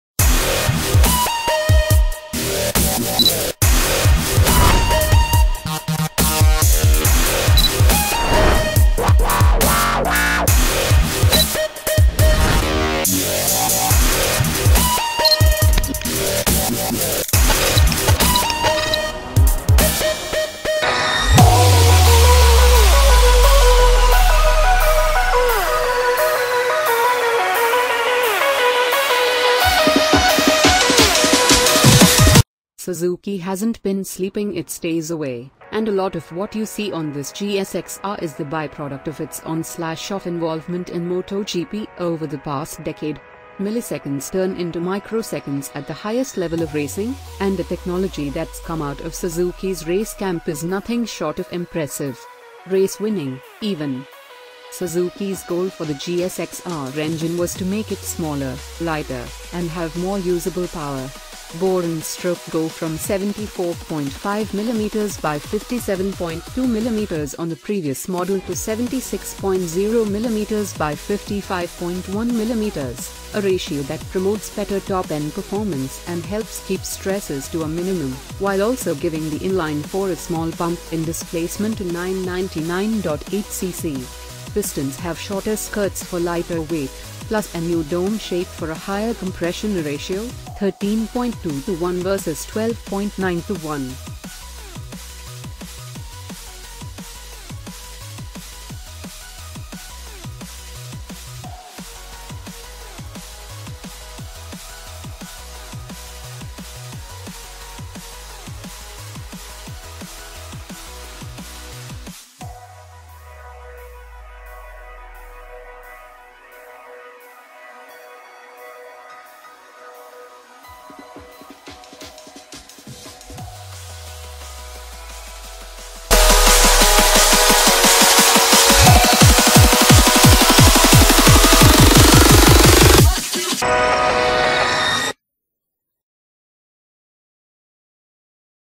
WOW AMAZING !!! 2017 Suzuki GSX R1000 ABS Price & Spec
WOW AMAZING !!! 2017 Suzuki GSX R1000 ABS Price & Spec - Suzuki hasn’t been sleeping its days away, and a lot of what you see on this GSX-R is the byproduct of its on/off involvement in MotoGP over the past decade. Milliseconds turn into microseconds at the highest level of racing, and the technology that’s come out of Suzuki’s race camp is nothing short of impressive. Race-winning, even.

$14,999